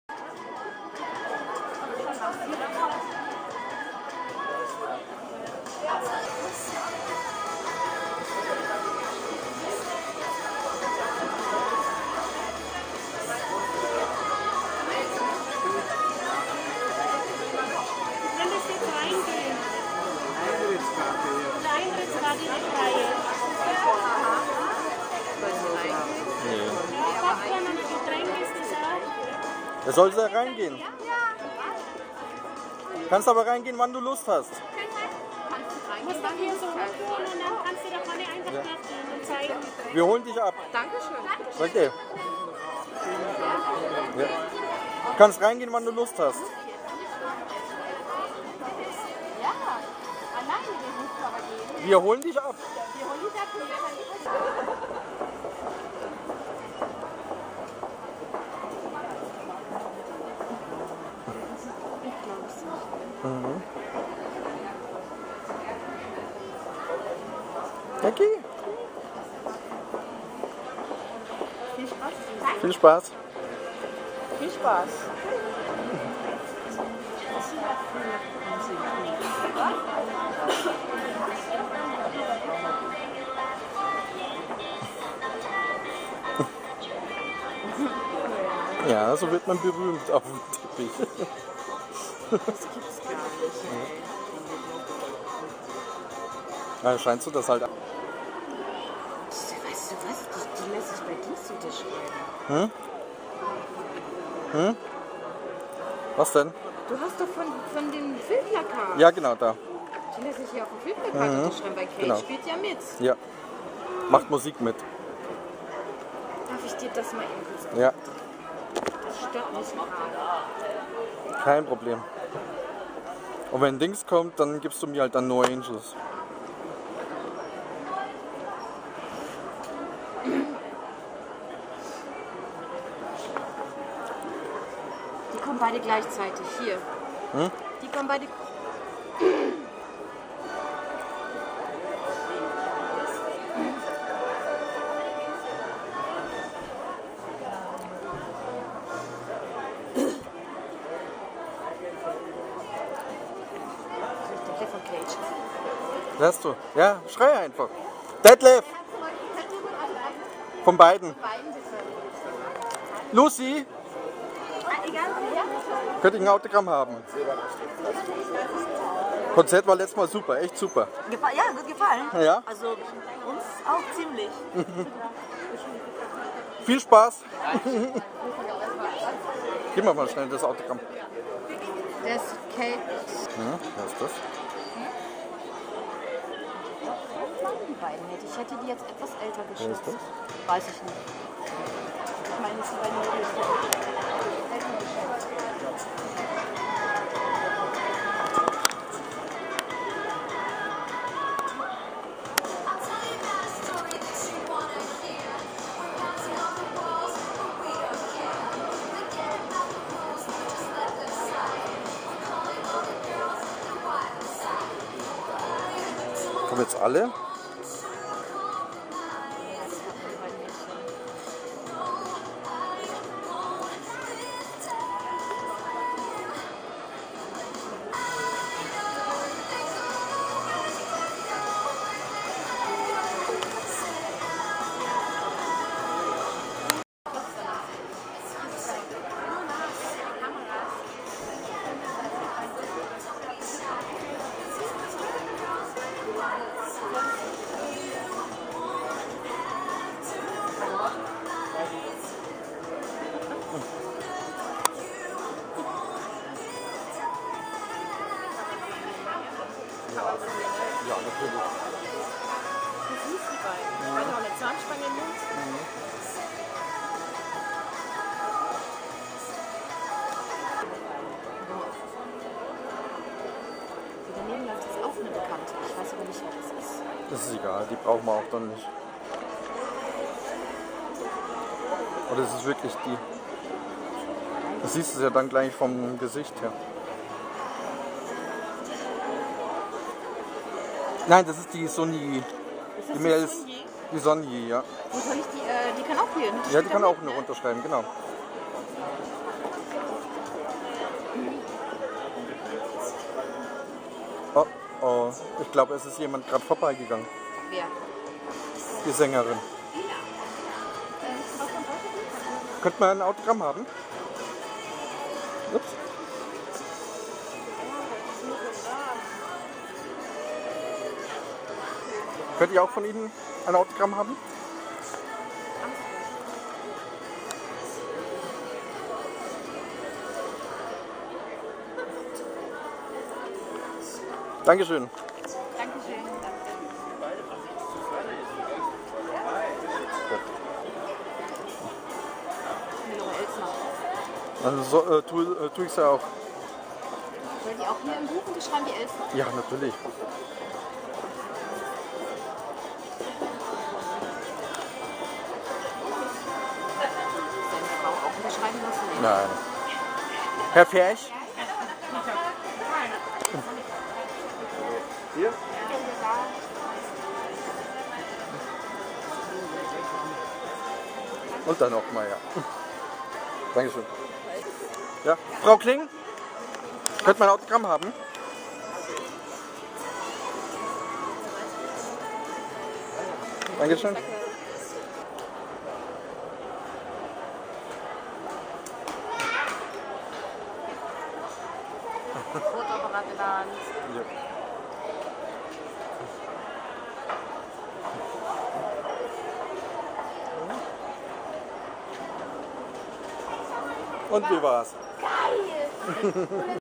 Weltpremiere Hanni & Nanni in München
Hanni & Nanni kommt ins Kino. Stars wie Anja Kling, Heino Ferch,Zwillinge Münster, D.Soost, Kate Hall, Lucy (No Angels) u.v.a. Unsere Tochter durfte dank eines Mannes und einer Frau auf den Teppich und ins Kino mit den Stars.